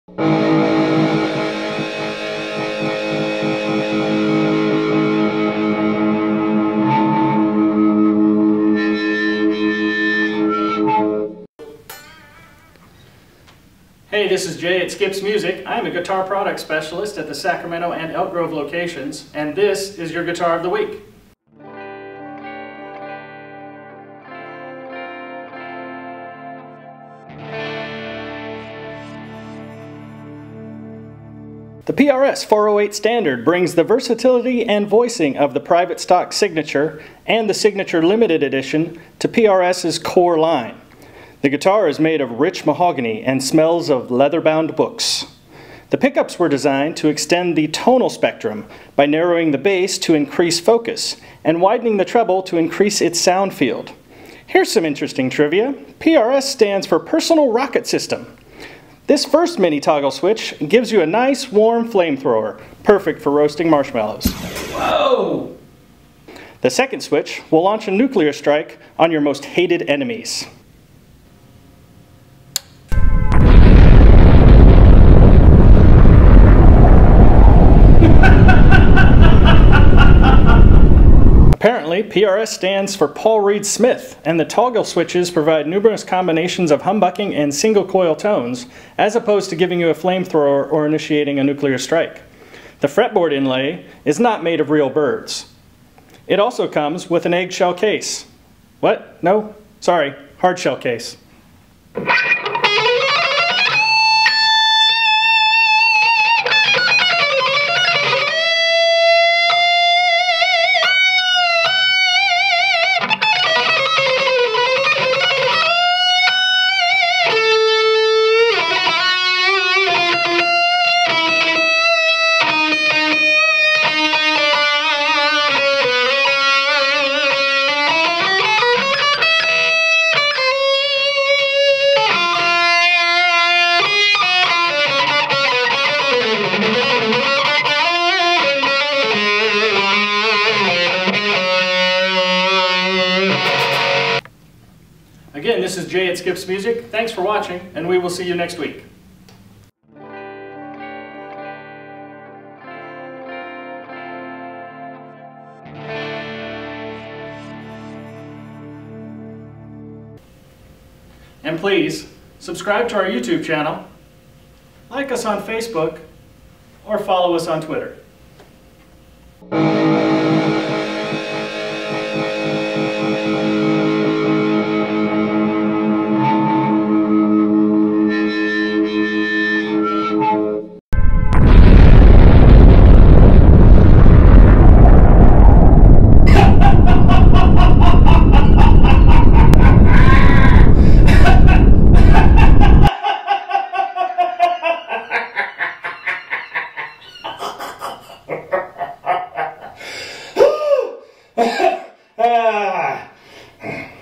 Guitar of the week 9
PRS 408

PRS is proud to offer the 408 Maple Top and 408 Standard. These inspiring instruments, offered as both a maple top (MT) and all mahogany (Standard) version, bring the versatility and voicing of the Private Stock Signature and the Signature Limited Edition to PRS’s Core line.
PRS 408 pickups were designed to extend the guitar’s tonal spectrum by narrowing the field of the bass pickup to increase its focus and widening the field of the treble pickup to increase its sound field (hence their asymmetric appearance). This pickup configuration (comprised of four coils and offering eight sounds) stems back to Paul’s experimentation on the Sorcerer’s Apprentice guitars in the early 1980’s. Thanks to the simple but versatile switching system featuring mini-toggle switches that provide numerous combinations of humbucking and singlecoil tones, the 408 Maple Top and 408 Standard provide stunning articulation and musical clarity.